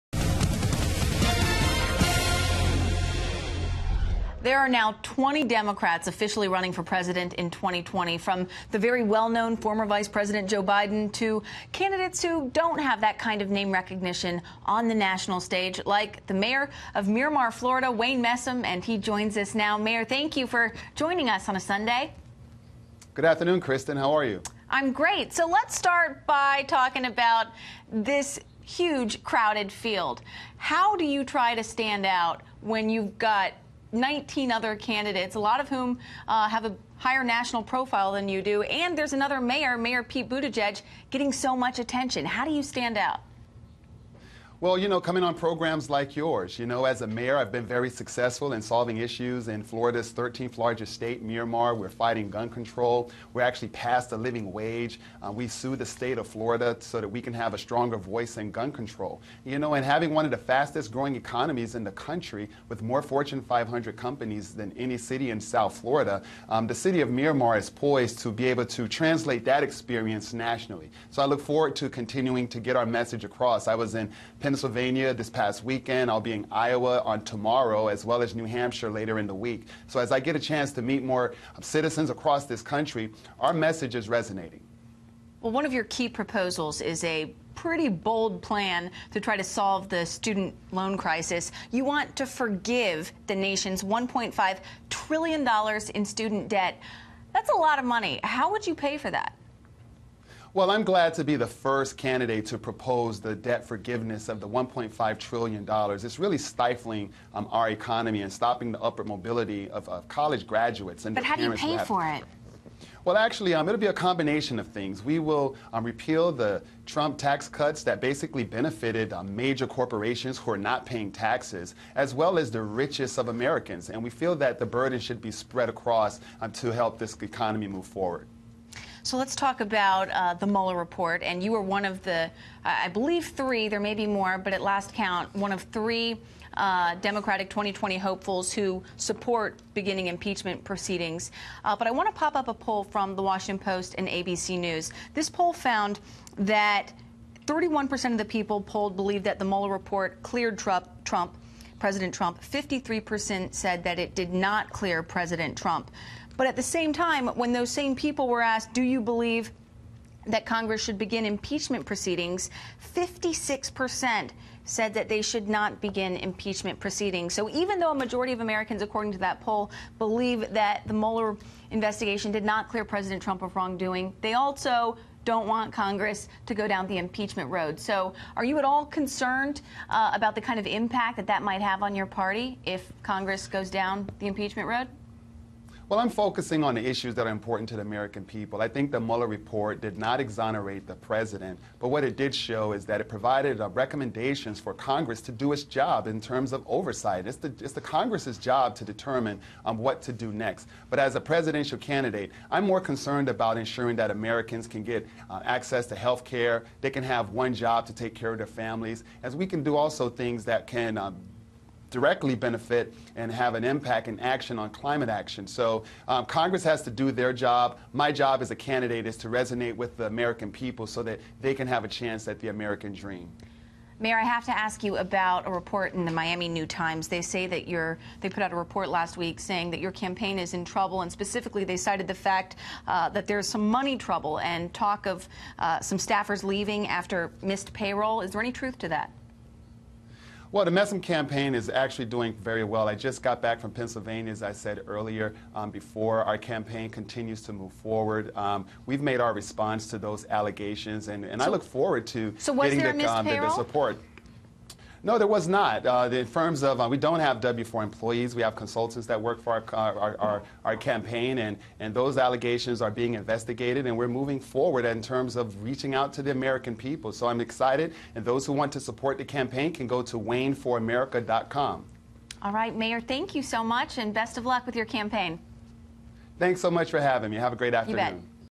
2020 Dem hopeful wants to forgive $1.5 trillion in student debt
2020 Dem hopeful wants to forgive $1.5 trillion in student debt